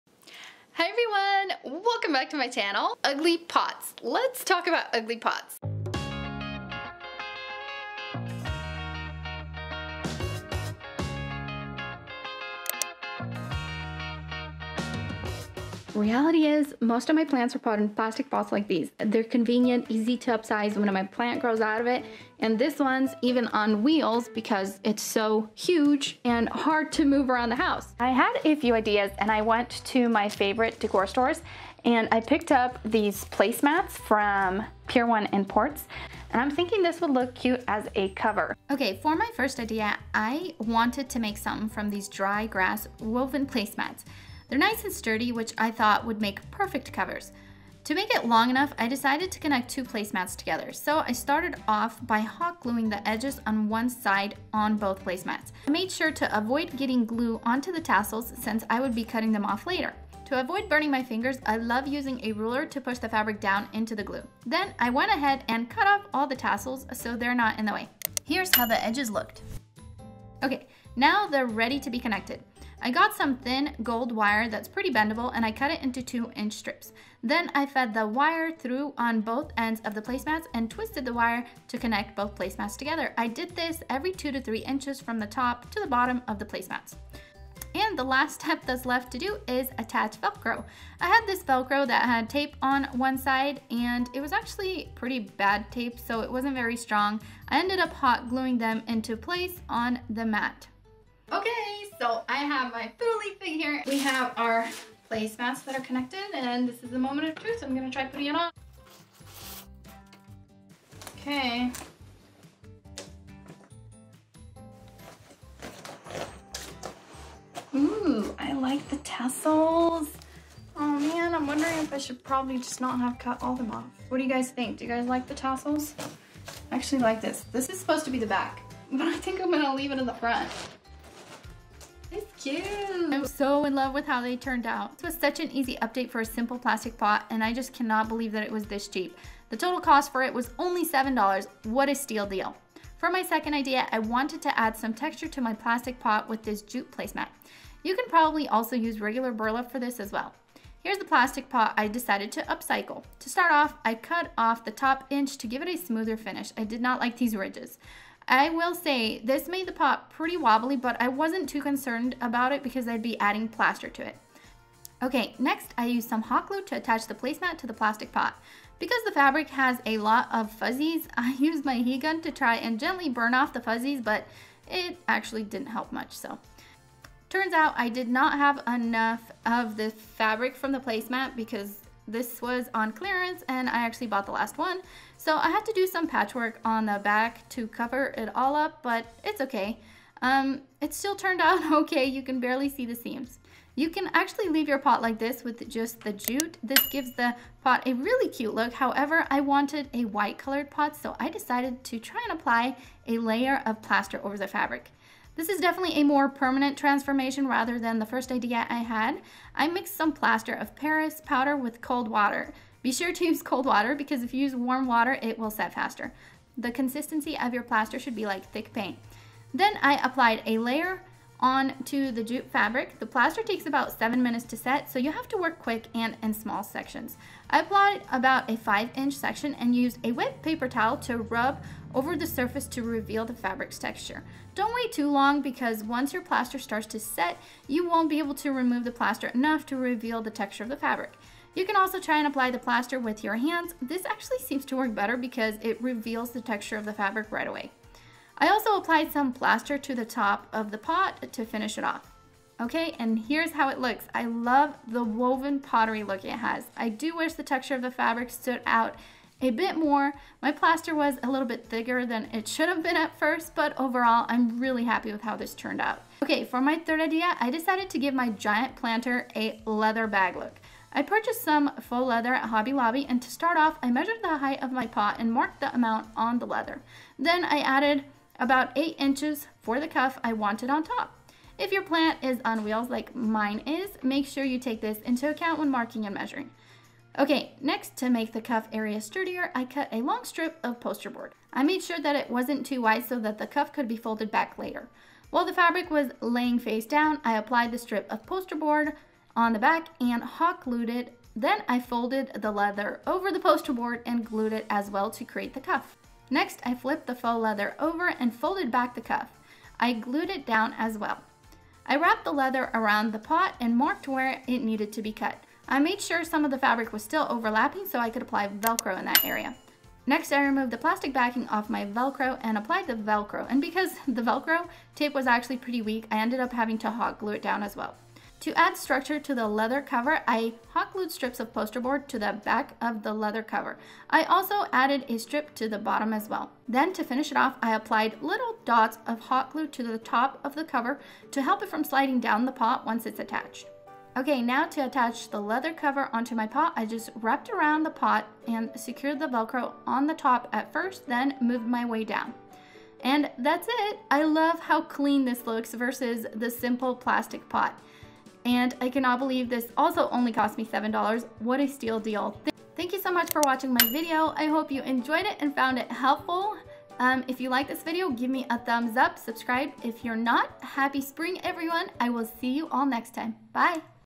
Easy Planter Pots HACK IN MINUTES! 3 Ways To Transform Ugly Plastic Pots Complete Makeover
Stay calm and craft on!

Hi Everyone! I wanted to share these quick plastic pot hacks. I call them ugly plastic pot covers! they give your ugly pots a complete makeover!  Each of them cost under $10 to make and 5-20 to complete.